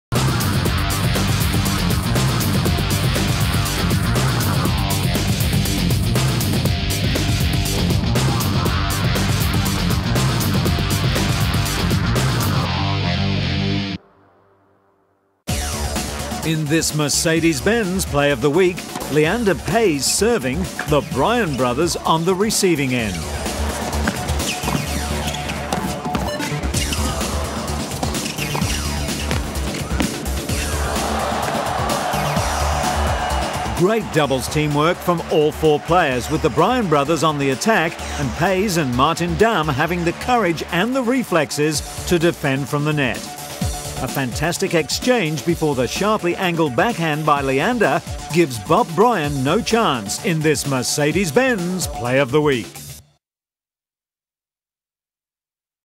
Paes-Damm doubles - fantastic!!!
This clip is from atptennis.com ...Superb volley and net play by Leander Paes n Martin Damm at the 2006 Shanghai Masters. Check Paes' finishing shot!!!